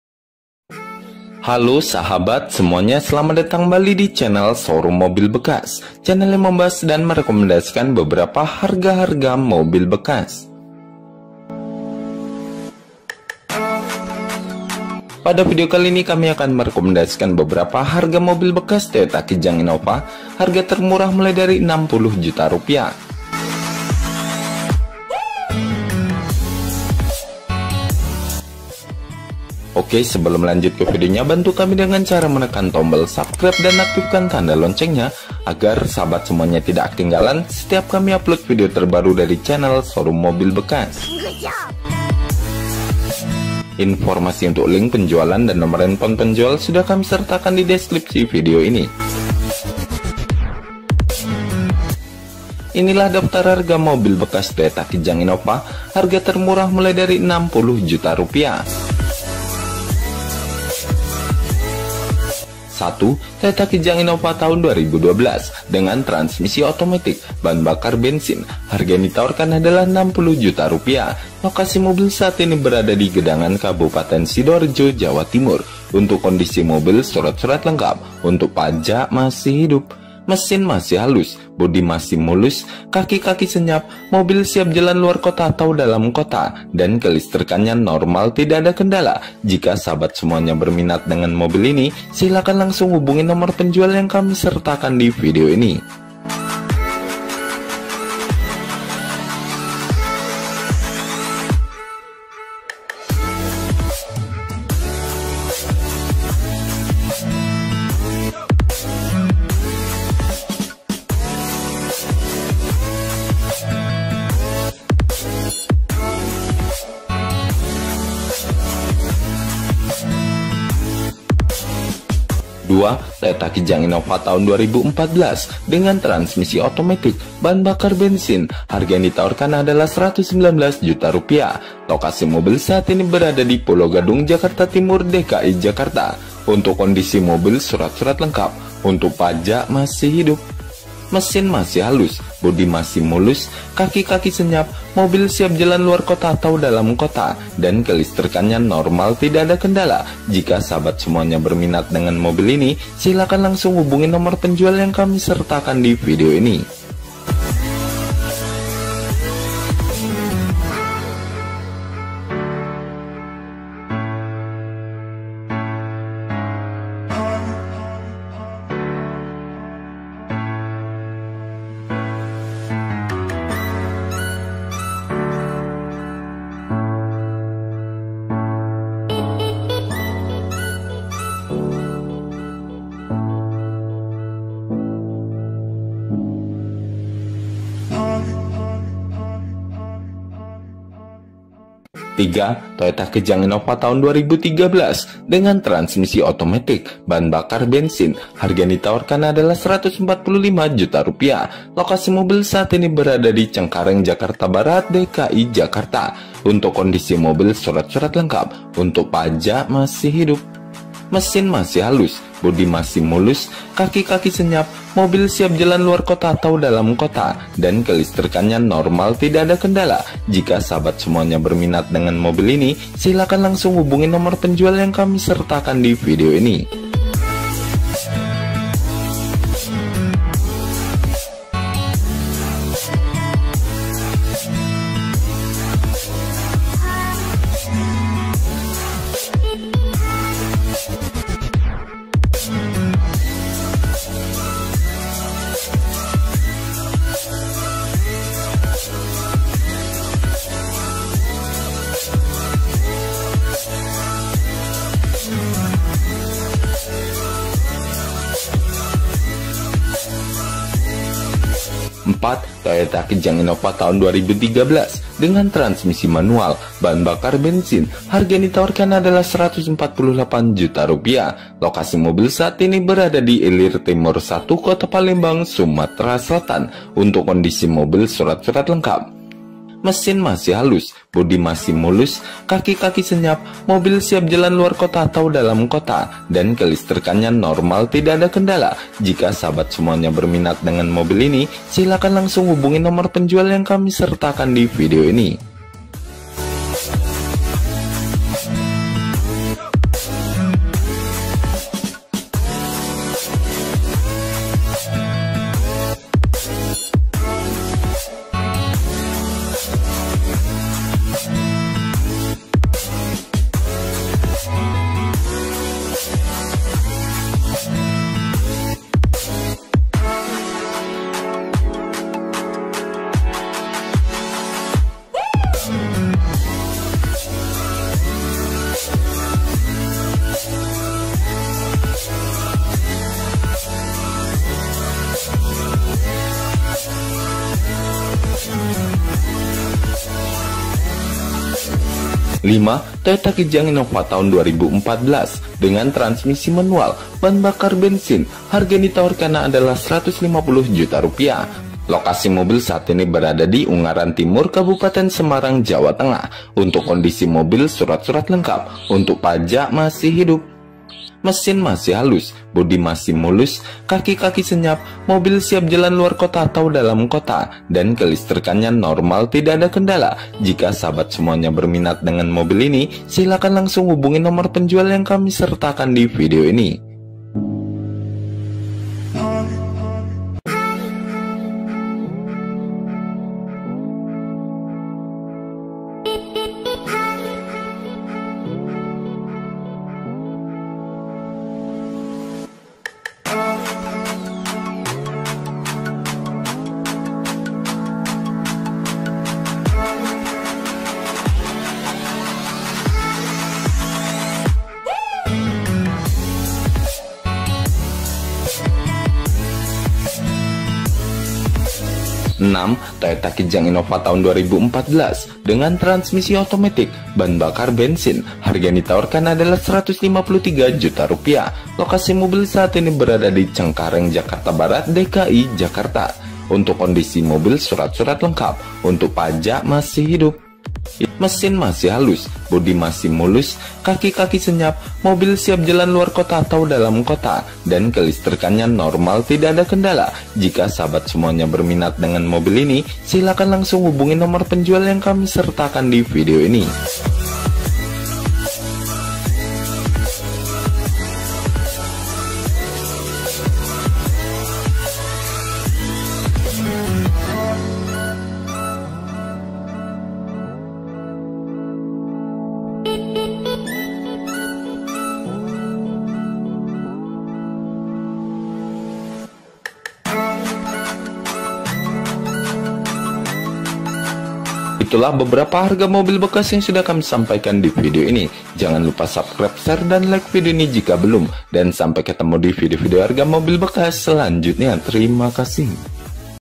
HARGA MOBIL TOYOTA KIJANG INNOVA 2012 HANYA 60 JUTA MURAH
HARGA MOBIL BEKAS TOYOTA KIJANG INNOVA

1. TOYOTA KIJANG INNOVA TAHUN 2012, HARGA 60 JUTA

2. TOYOTA KIJANG INNOVA TAHUN 2014, HARGA 119 JUTA

3. TOYOTA KIJANG INNOVA TAHUN 2013, HARGA 145 JUTA

4. TOYOTA KIJANG INNOVA TAHUN 2013, HARGA 148 JUTA

5. TOYOTA KIJANG INNOVA TAHUN 2014, HARGA 150 JUTA

6. TOYOTA KIJANG INNOVA TAHUN 2014, HARGA 153 JUTA

TERIMA KASIH